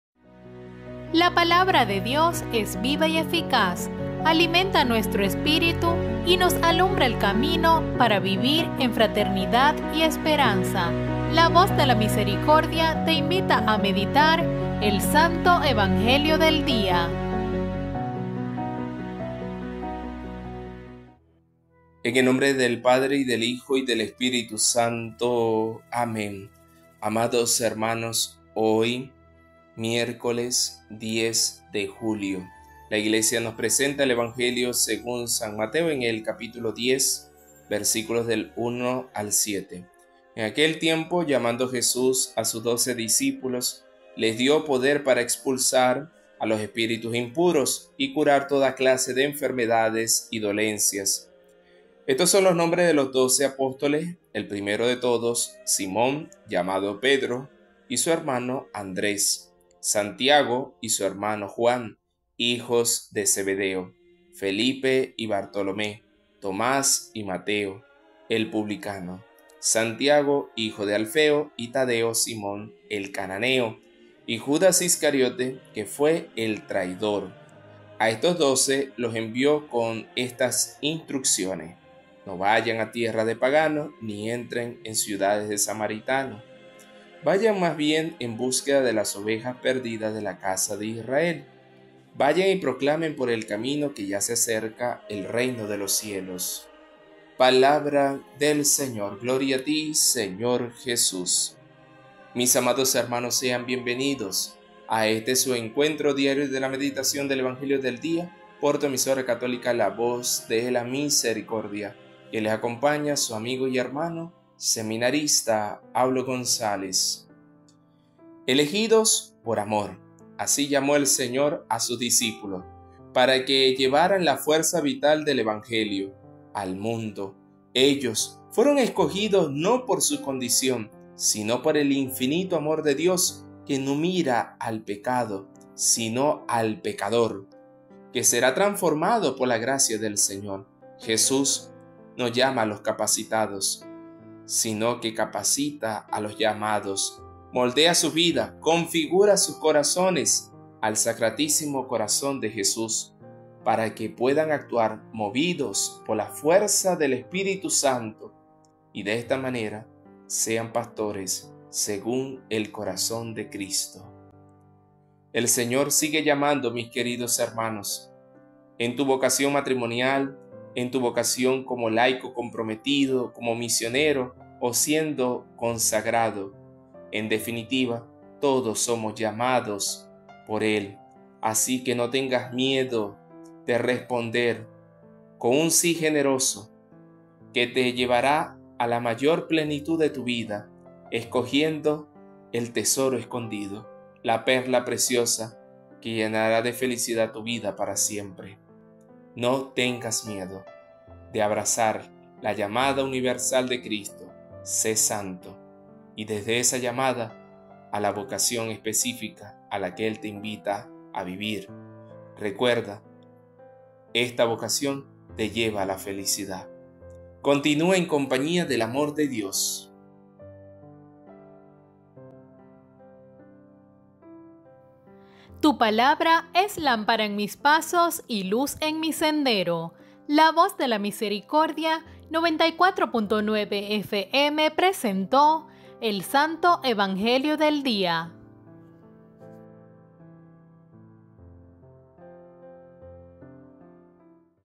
Santo Evangelio del Día (10/07/2024) San Mateo 10, 1 - 7. Seminarista Aulo González.
[1]Jesús llamó a sus doce discípulos y les dio poder sobre los malos espíritus para expulsarlos y para curar toda clase de enfermedades y dolencias.
[2]Estos son los nombres de los doce apóstoles: primero Simón, llamado Pedro, y su hermano Andrés; Santiago, hijo de Zebedeo, y su hermano Juan;
[3]Felipe y Bartolomé; Tomás y Mateo, el recaudador de impuestos; Santiago, el hijo de Alfeo, y Tadeo;
[4]Simón, el cananeo y Judas Iscariote, el que lo traicionaría.
[5]A estos Doce Jesús los envió a misionar, con las instrucciones siguientes: «No vayan a tierras de paganos, ni entren en pueblos de samaritanos.
[6]Diríjanse más bien a las ovejas perdidas del pueblo de Israel.
[7]A lo largo del camino proclamen: ¡El Reino de los Cielos está ahora cerca!